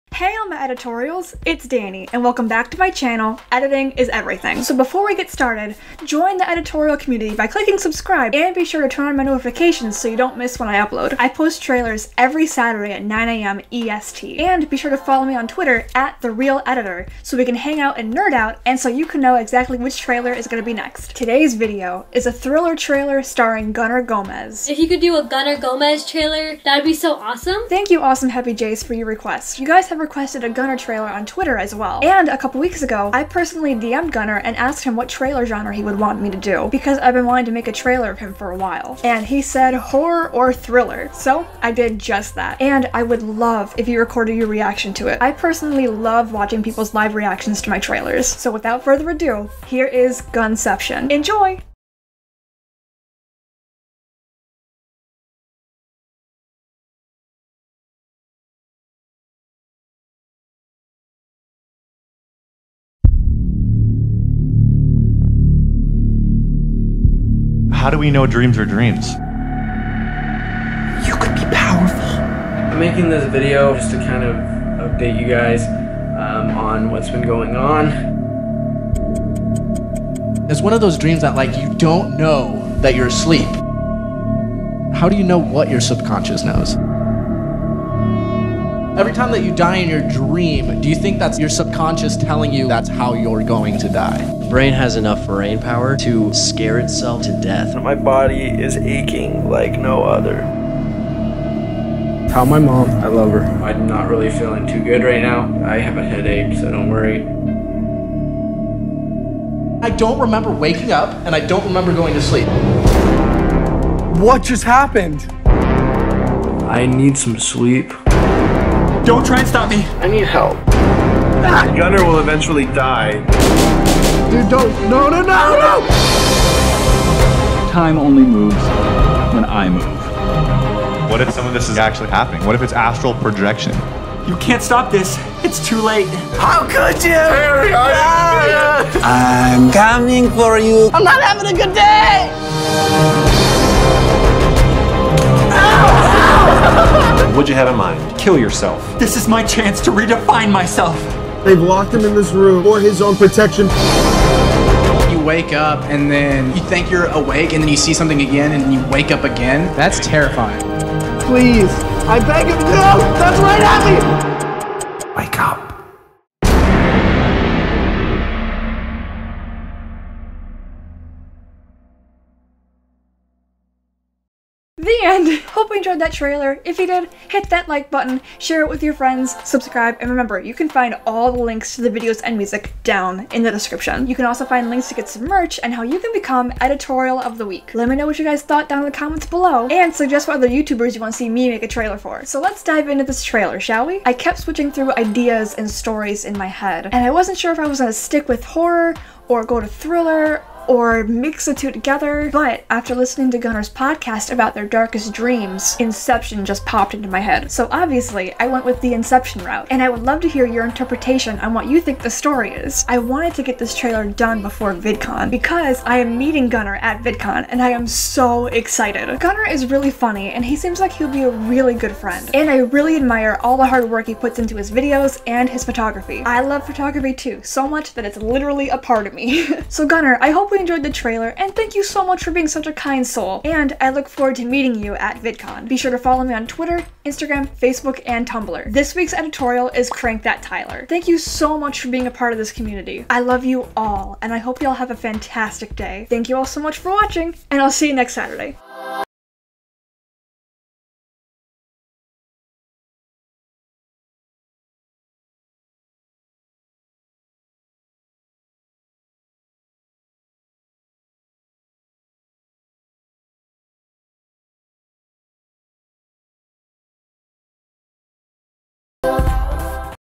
GUNCEPTION | Thriller Trailer (Gunner Gomez)
In the year 2048, a new technology allowed willing patients to delve deeper into their dreams, but when some patients go too deep, they can't wake up. Stuck in their dreams, they don't know what is real and what is not. Use the hashtag EditingIsEverything to suggest what I should do next!

Click here to find out how to become an Editorial of the Week!

VIDEO AT 2:35